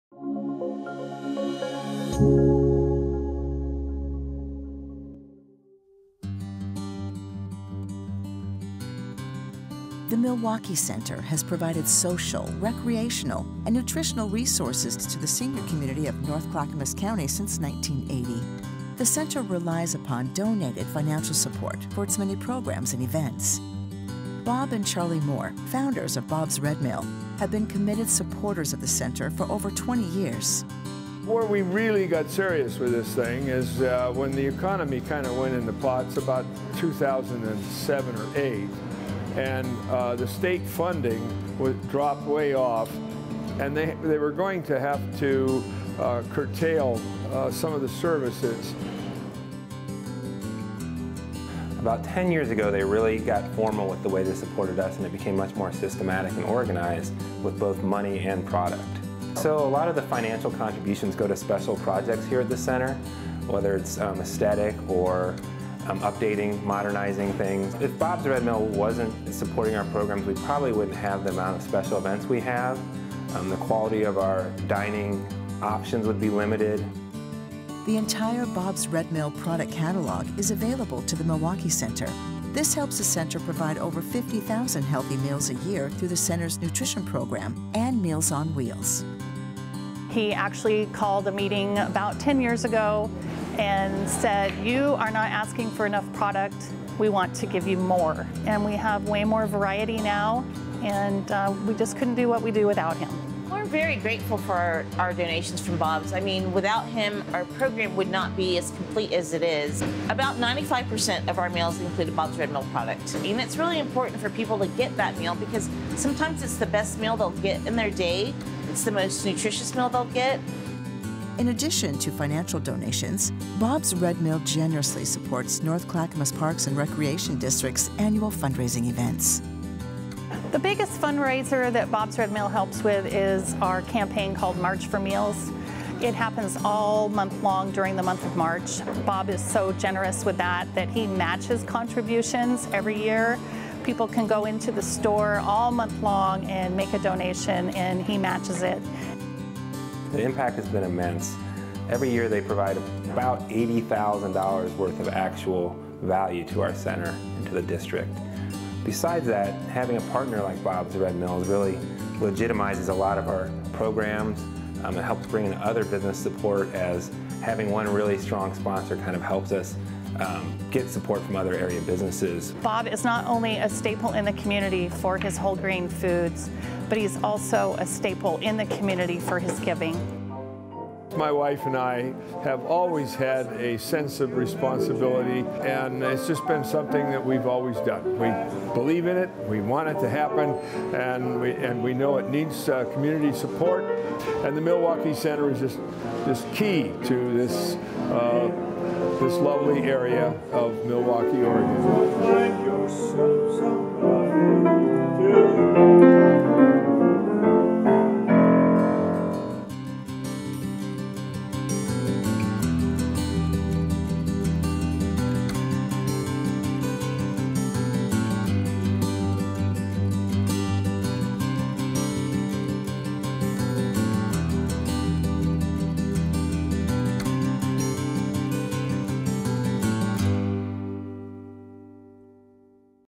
Bob's Red Mill Supporting the Milwaukie Center & Community
See how Bob's Red Mill supports the Milwaukie Center's programs like Meals on Wheels and the impact those programs have on the North Clackamas County community.